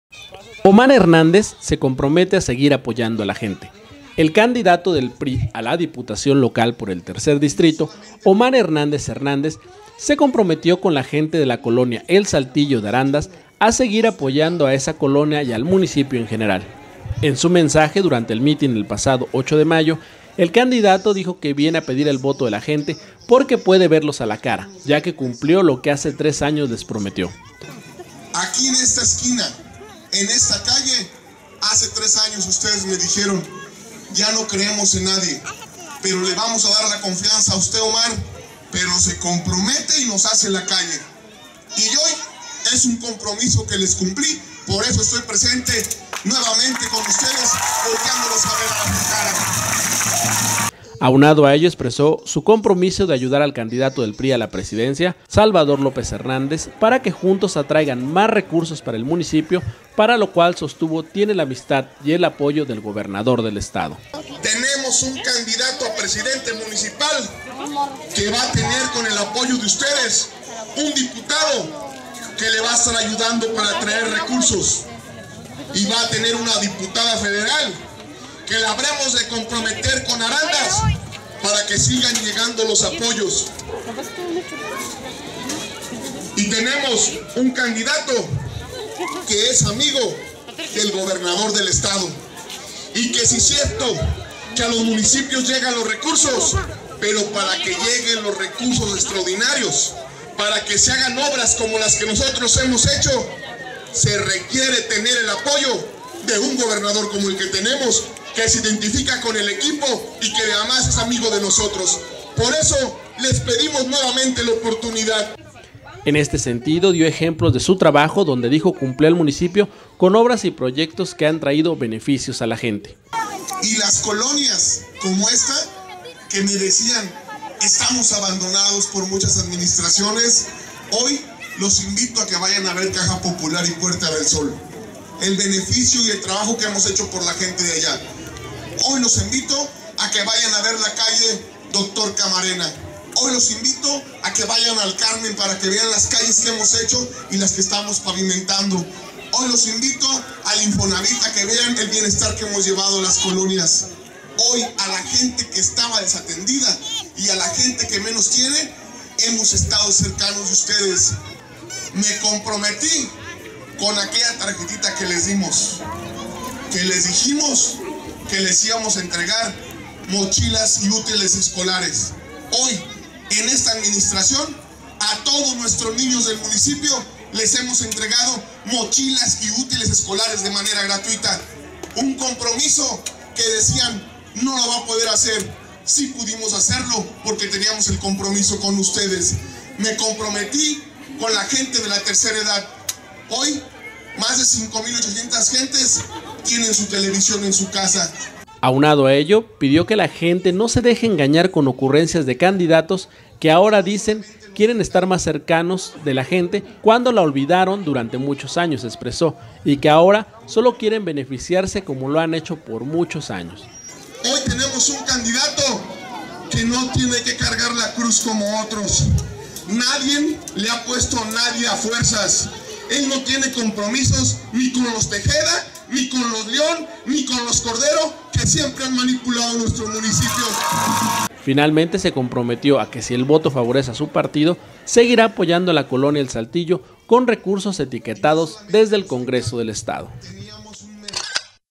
Visita a la colonia El Saltillo, en Arandas, Omar Hernández
En su visita a la colonia El Saltillo, en Arandas, Omar Hernández candidato a la diputación local por el 3er distrito se compromete ayudar a la gente como afirma ya lo ha hecho.